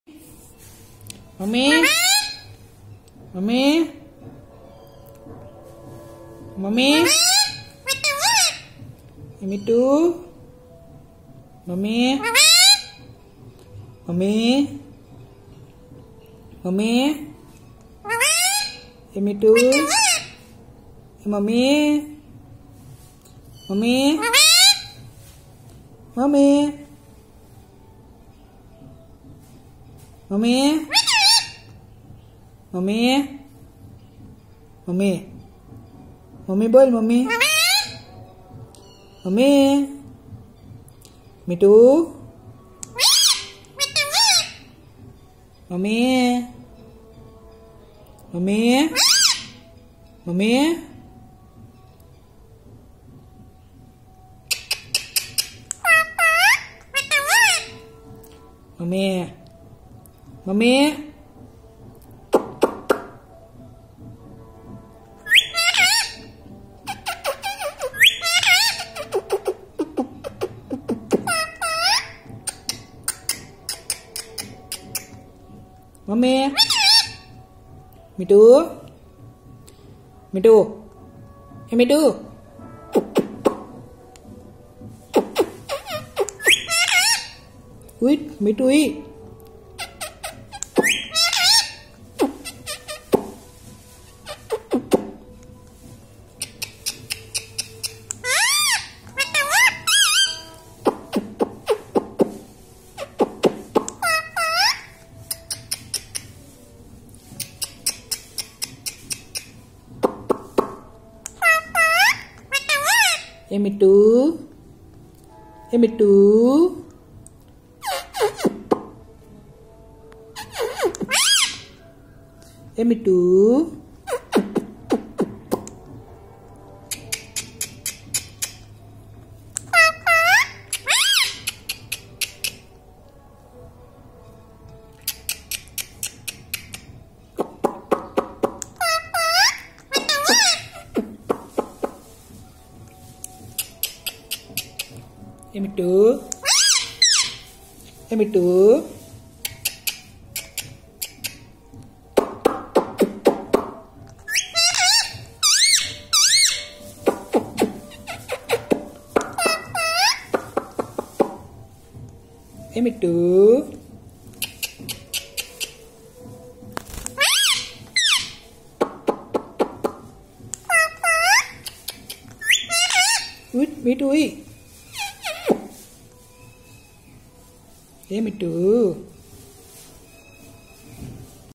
बोलने वाला प्यारा तोता - (ツ) talking parrot mittu
mama bolne wala baby

बोलने वाला प्यारा तोता
चोटी मिट्ठू की प्यारी बाते
मिट्ठू बोलने वाला प्यारा तोता
सीता राम,सीता राम बोलने वाला अद्भुत तोता